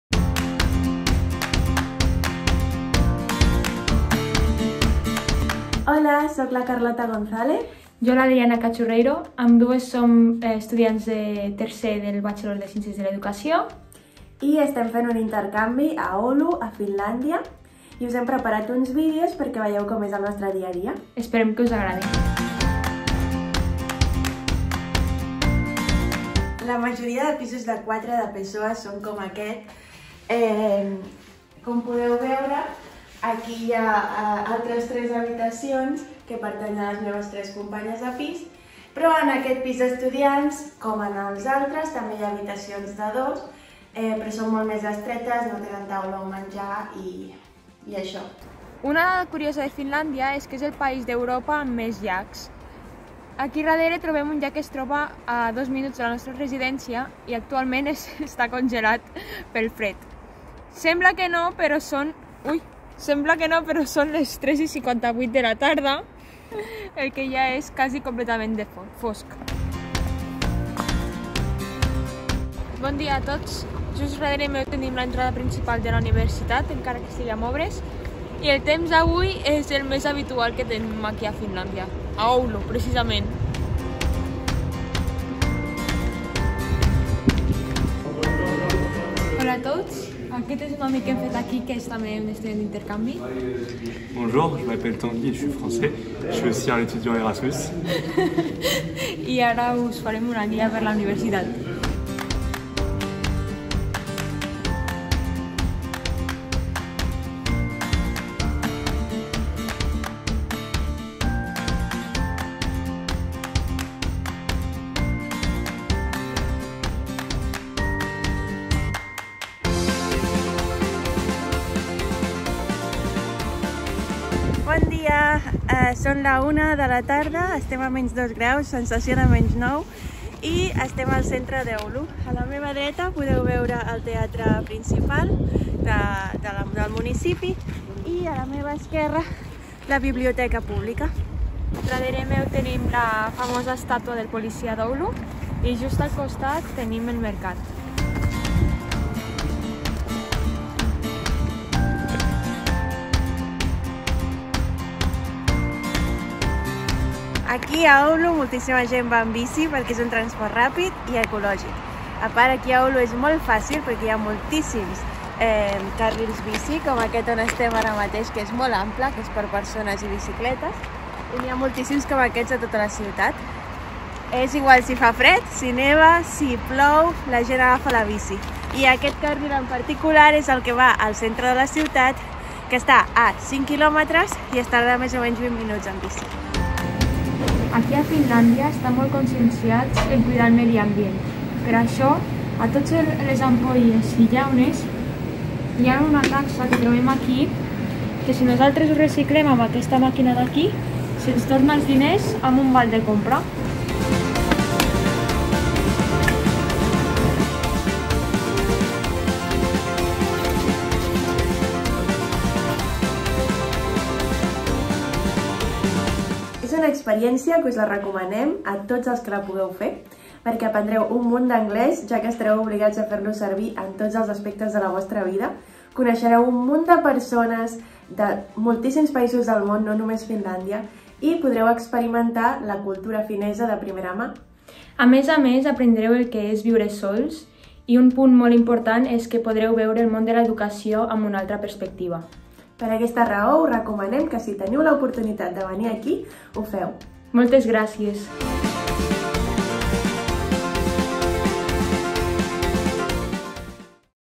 Experiència de mobilitat a Finlàndia
Les estudiants del bàtxelor en Ciències de l'educació Carlota Gonzàlez i Adriana Cachurreiro expliquen la seva experiència fent un semestre de mobilitat internacional a la Universitat d'Oulu, a Finlàndia.
Ets estudiant de la Universitat d'Andorra i vols cursar un semestre a l'estranger?